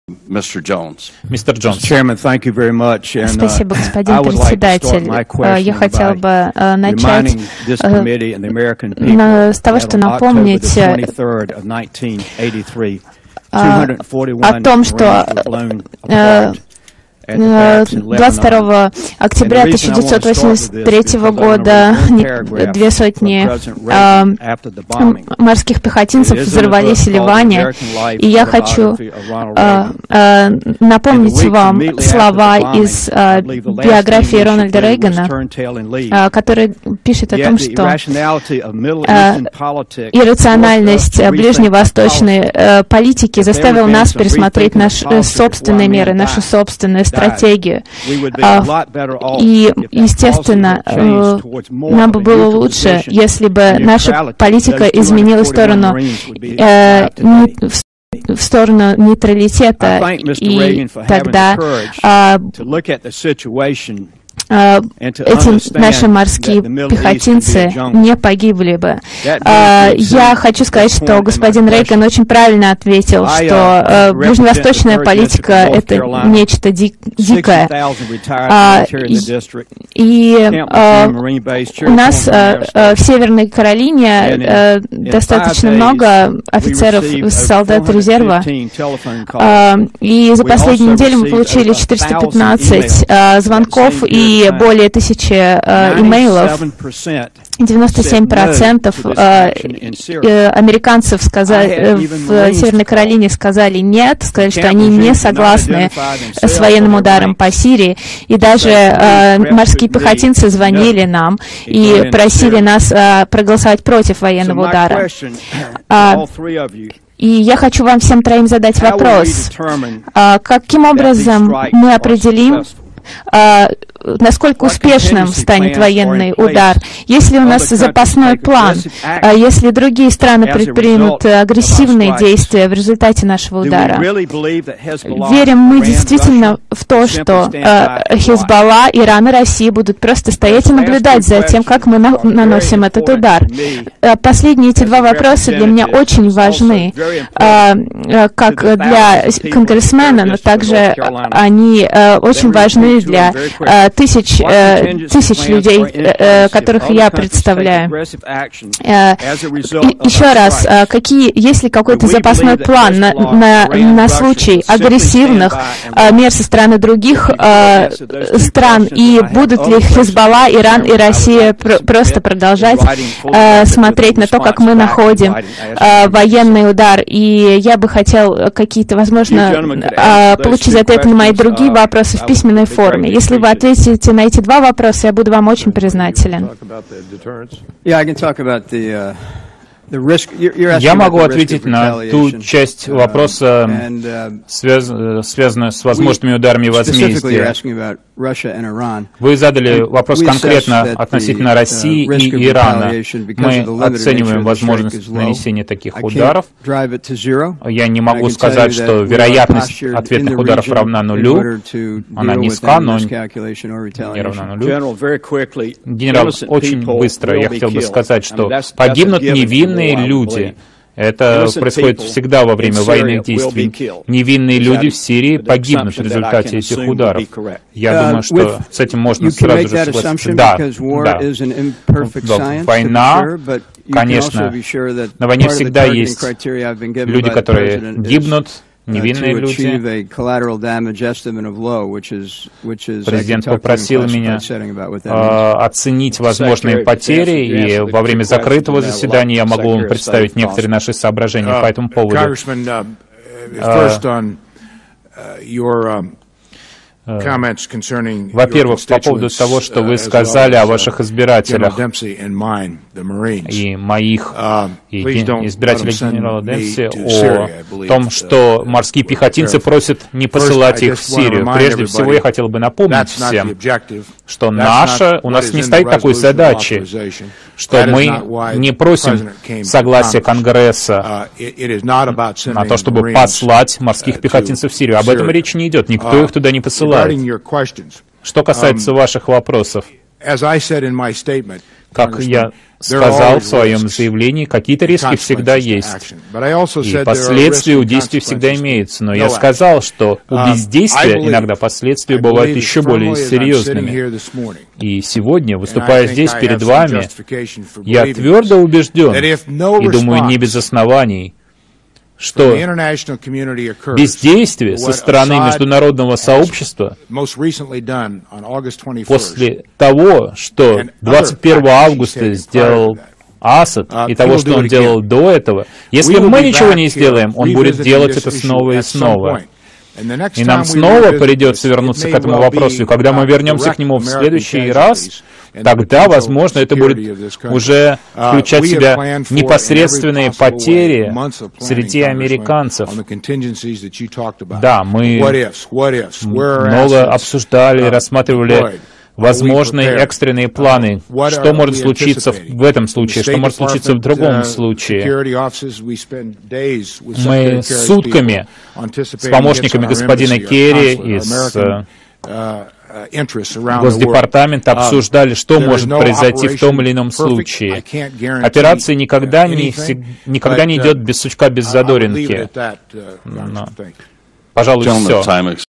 Сирия. морпехи США против военного удара - слушания в Конгрессе 10.09.2013
"...хотел бы напомнить о том, что 22 октября 1983 года две сотни морских пехотинцев подорвались в Ливане. И я хочу напомнить вам слова из биографии Рональда Рейгана, который пишет о том, что иррациональность ближневосточной политики заставила нас пересмотреть свой собственный мир и свою собственную стратегию.  И естественно, нам бы было лучше, если бы наша политика изменилась в сторону нейтралитета, и тогда эти наши морские пехотинцы не погибли бы. Я хочу сказать, что господин Рейган очень правильно отметил, что ближневосточная политика - это нечто дикое.
И у нас в Северной Каролине достаточно много офицеров и солдат резерва. И за последнюю неделю мы получили 415 звонков и более тысячи писем. 97 %  американцев в Северной Каролине сказали "нет", сказали, что они не согласны  с военным ударом по Сирии. И даже морские пехотинцы звонили нам, и просили нас проголосовать против военного удара".
"И я хочу вам всем троим задать вопрос:  каким образом мы определим, насколько успешным станет военный удар.Есть ли у нас запасной план, если другие страны предпримут агрессивные действия в результате нашего удара? Верим мы действительно в то, что Хизболла,Иран и Россия будут просто стоять и наблюдать за тем, как мы наносим этот удар? Последние два вопроса для меня очень важны, как для конгрессмена, но также они очень важны для тысяч, тысяч людей, которых я представляю", -  конгрессмен от штата Северная Каролина Уолтер Джонс (Walter Jones)
JONES OPPOSES OBAMA ON SYRIA

фрагмент трансляции выступления госсекретаря США Джона Керри, министра обороны Чака Хейгела и председателя Объединенного комитета начальников штабов Мартина Демпси перед Комитетом Вооружённых сил США по военной интервенции в Сирии. 1ё0 сентября 2013 года

Кнгрессмен Walter Jones опубликовал следующее заявление относительно резолюции, которая санкционирует военное вмешательство США в Сирии:
"Я буду голосовать против любых предложение разрешить военное вмешательство США в Сирии.
... есть серьезные сомнения в достоверности претензий администрации Обамы в отношении использования газа зарина в Сирии. Президент не сумел объяснить, как наша национальная безопасность зависит от участия в еще одной ближневосточной гражданской войне на стороне радикальных исламистских повстанцев, которые были зарегистрированы как обладающие газ зарин и обезглавливание христиан, среди других злодеяний ".